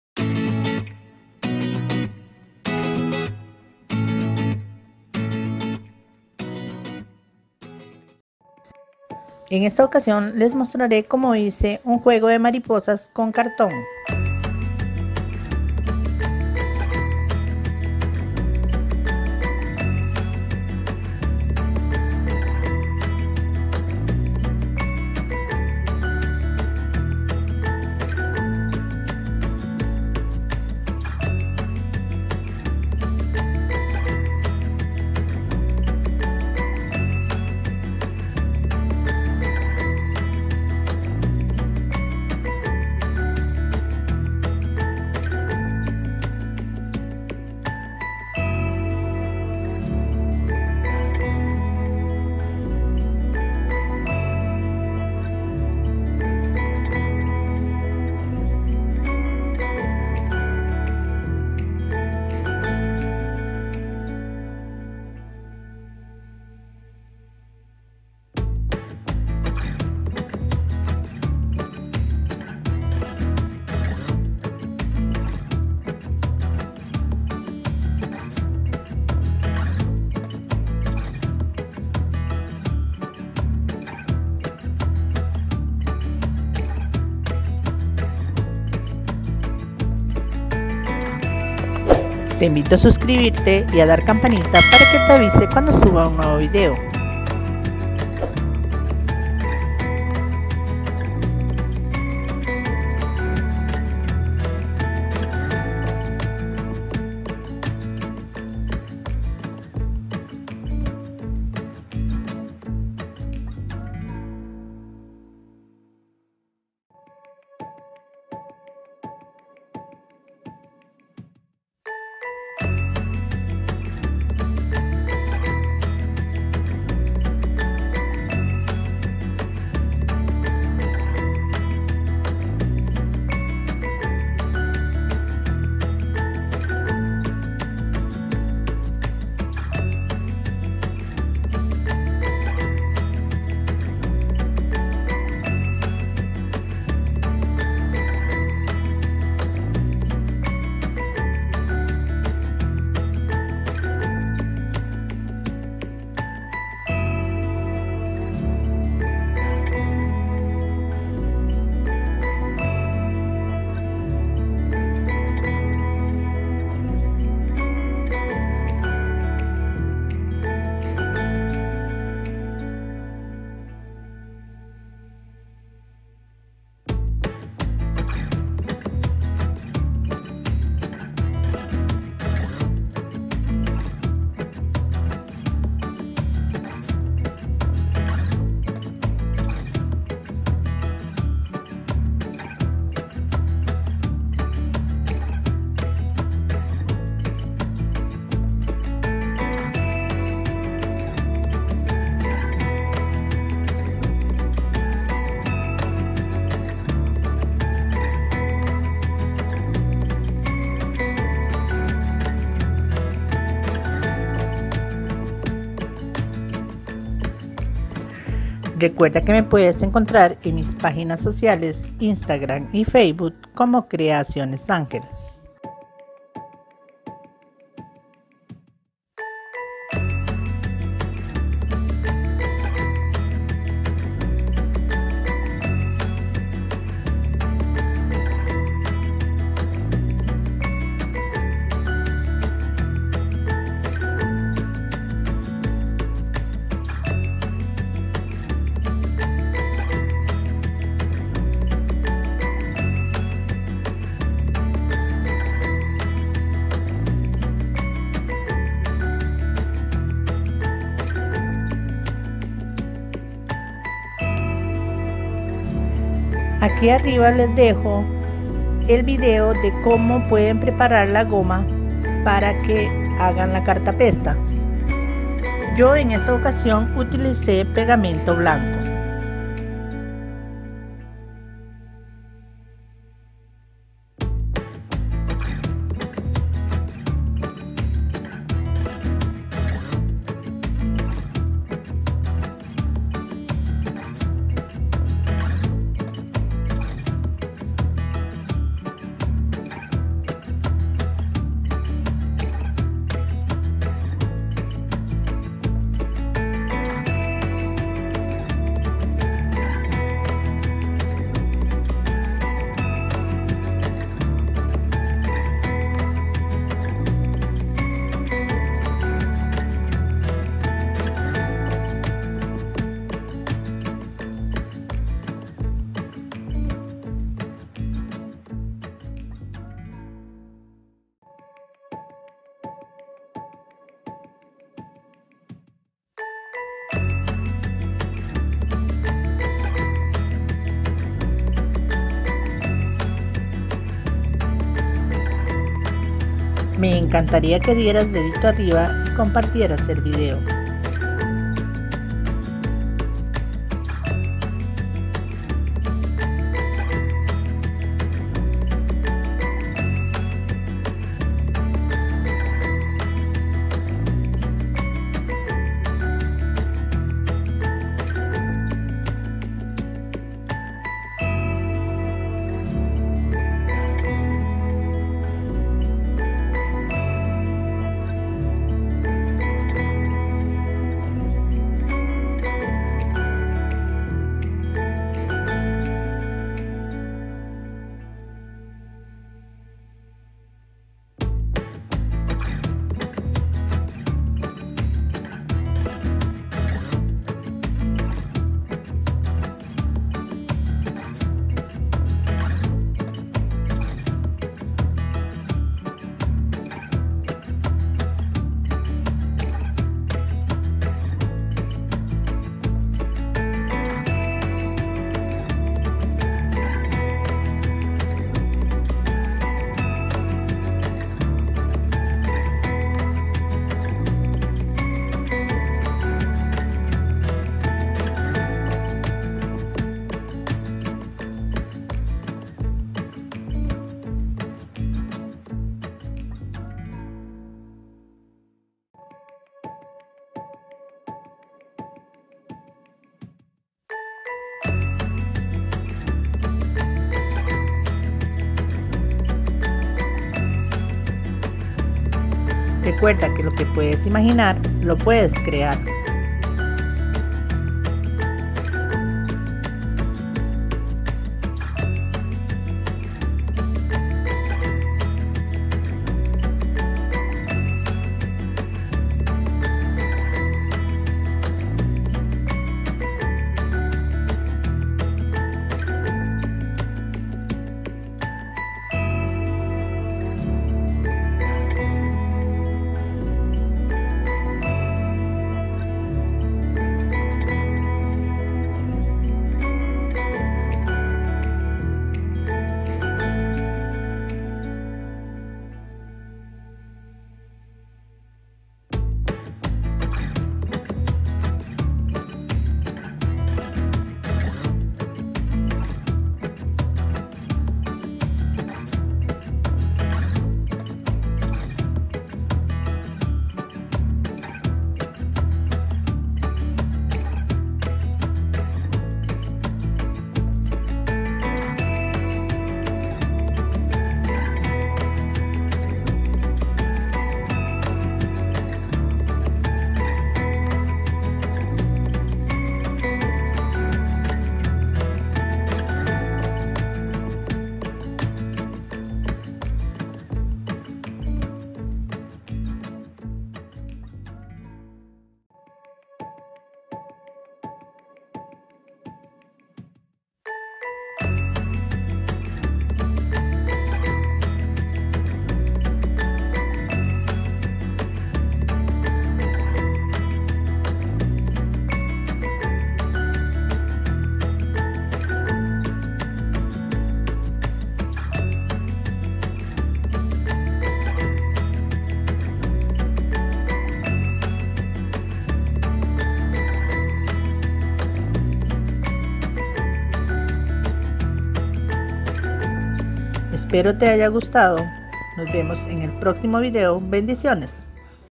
COMO HACER MARIPOSAS DE CARTÓN
MATERIALES
CARTÓN / CUTER
SILICON CALIENTE /CINTA DE CARPINTERO
PLANTILLA  / PAPEL PERIODICO O REVISTAS
COLA BLANCA  O GOMA DE LA QUE HICE
PILOT / PINTURAS ACRILICAS O DE AGUA
OINCEL Y UNA TIRITA PARA GUINDARLAS